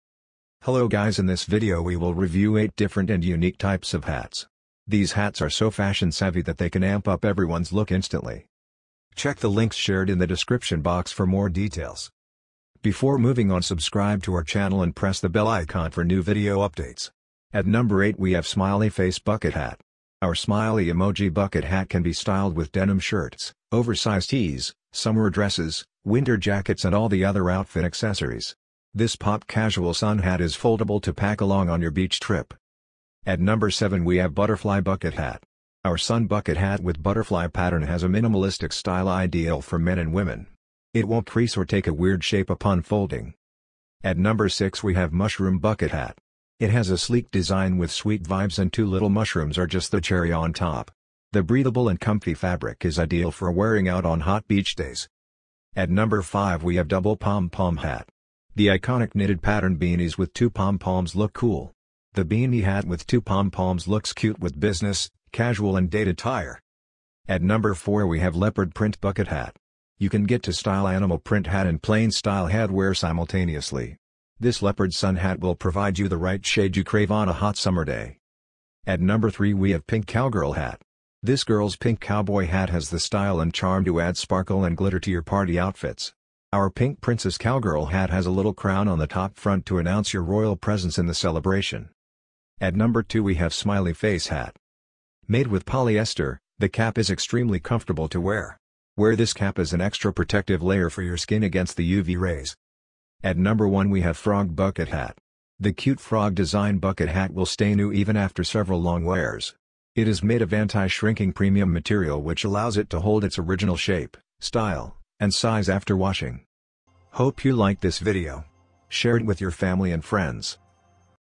8 Different & Unique Types Of Hats Every Fashion Lover Should Have
These are the unique and different types of hats you can wear all day to let others know how fashion-savvy you are: